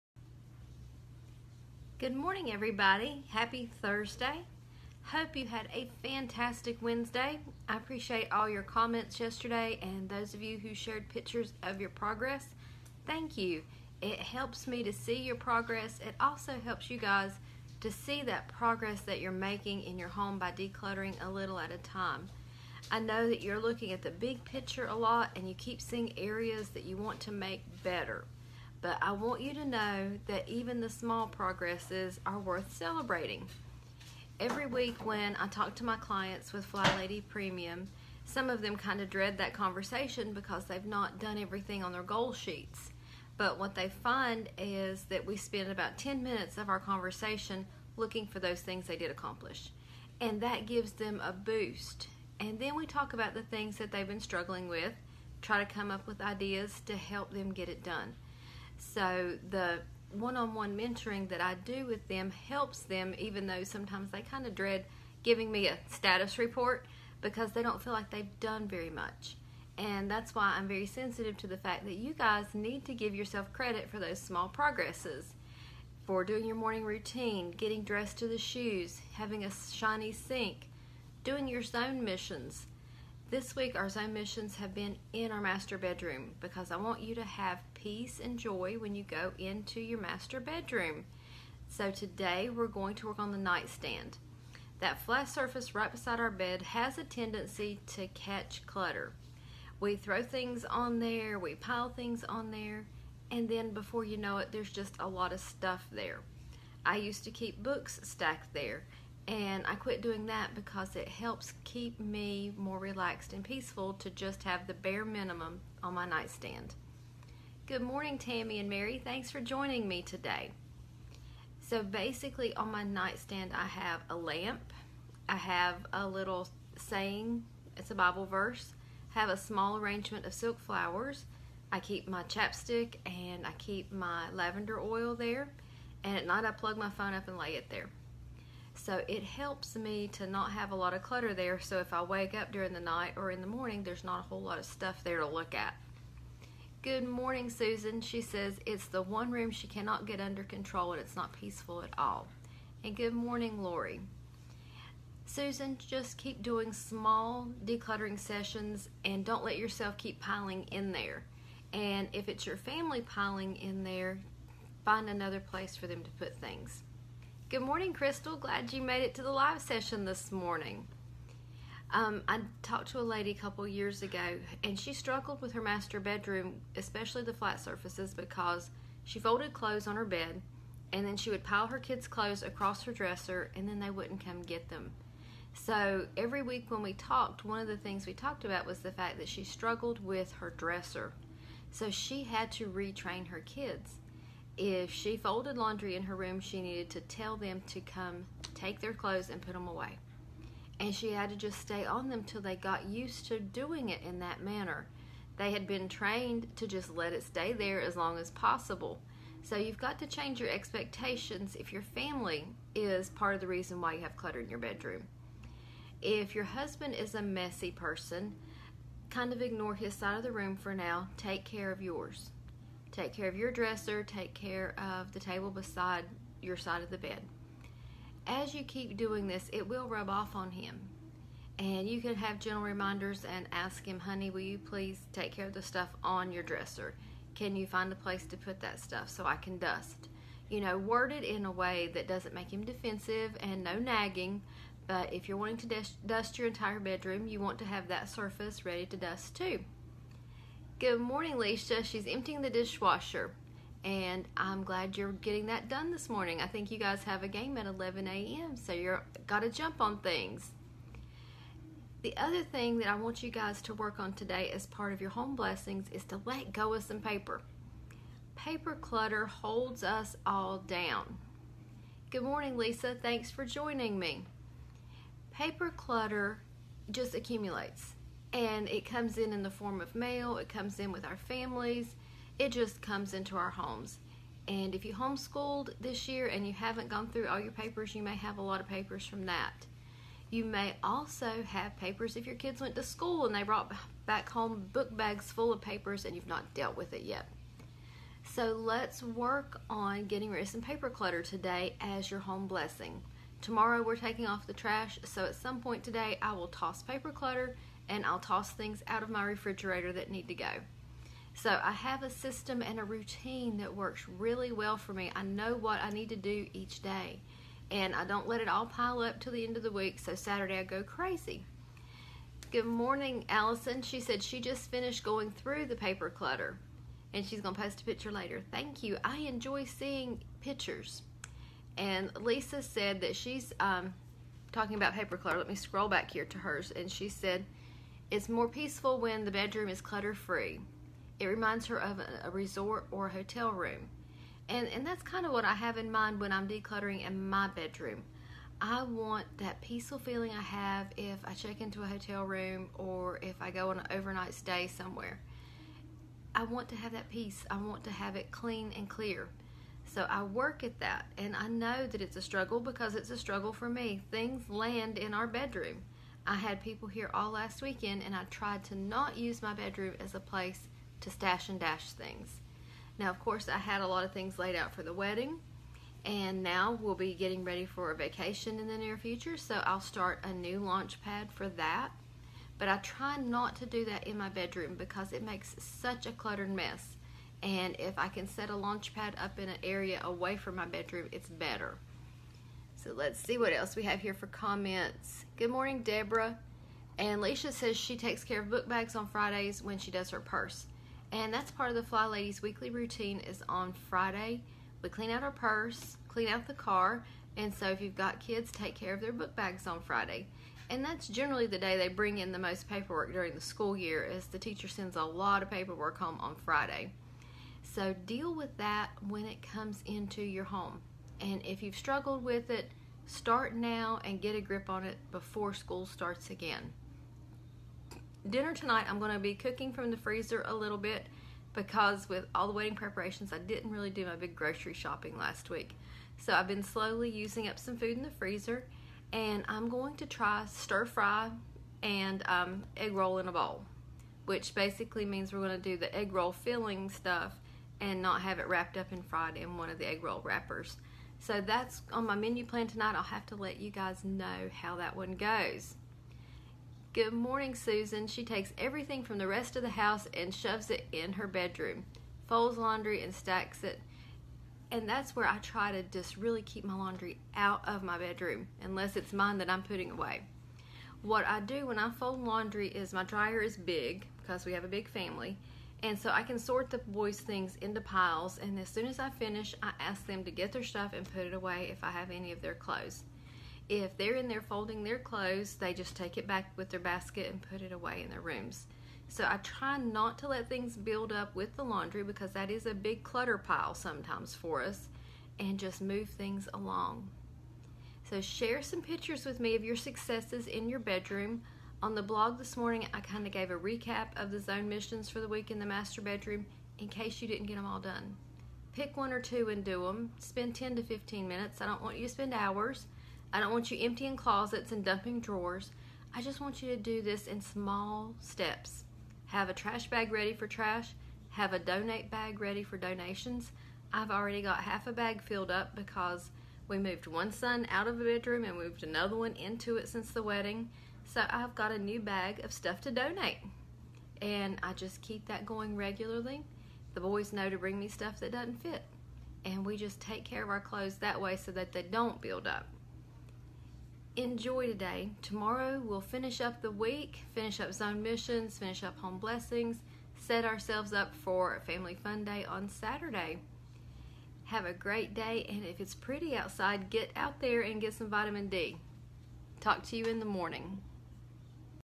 Bringing Peace and Happiness to your Bedroom
Doing your weekly zone missions in the master bedroom will help you have peace and happiness in your bedroom.